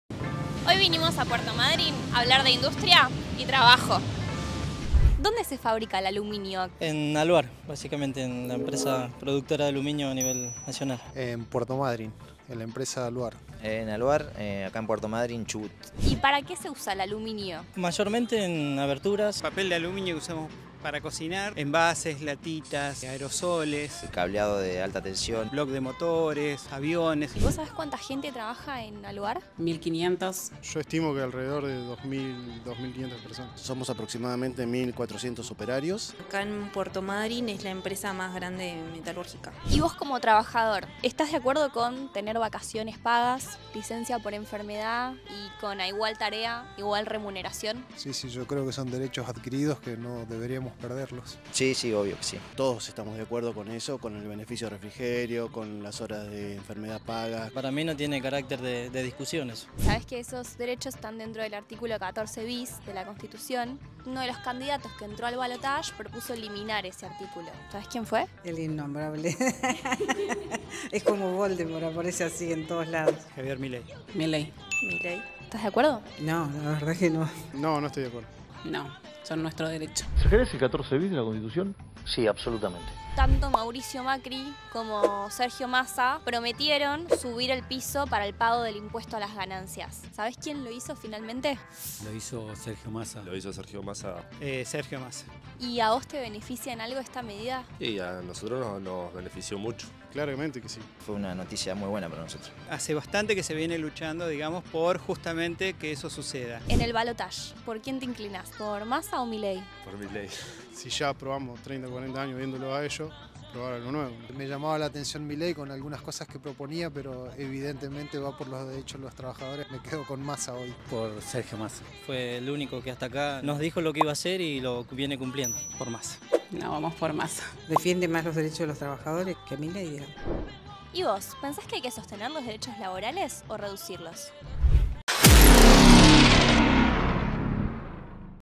Hoy les mostramos ésta encuesta e información desde Puerto Madryn.
Una encuesta realizada con operarios y residentes en La Ciudad de Puerto Madryn.